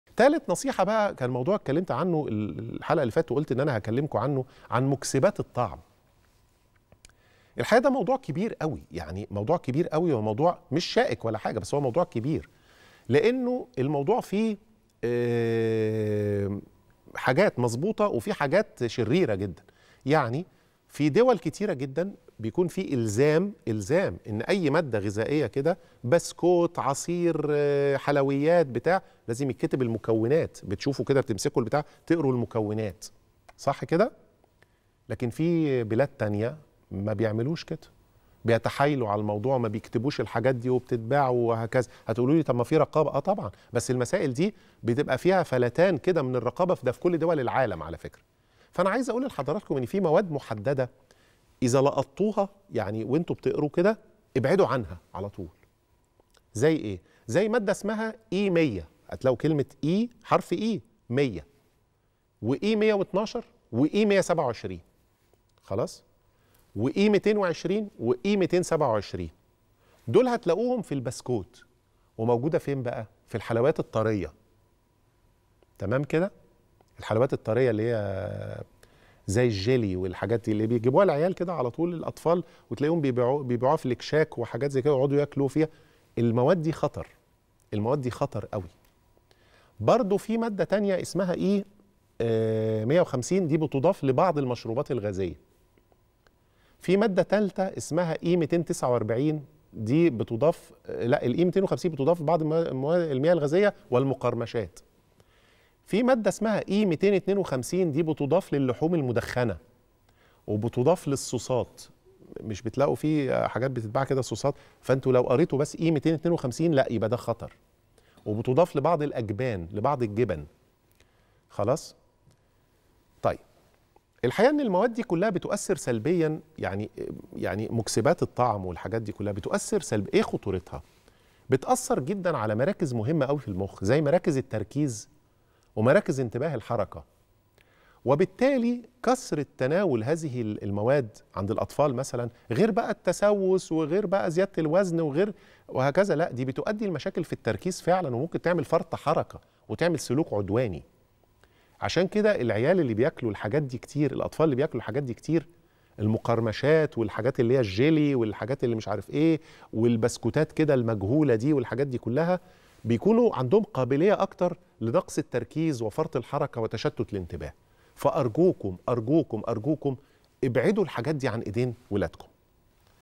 في البسكويت.. مادة خطيرة لو لقيتها في أحد مكونات الأطعمة ابعد عنها فوراً..تعرف عليها مع د. محمد رفعت
يحكى أن | "في البسكويت".. مادة خطيرة لو لقيتها في أحد مكونات الأطعمة ابعد عنها فوراً.. تعرف عليها مع د. محمد رفعت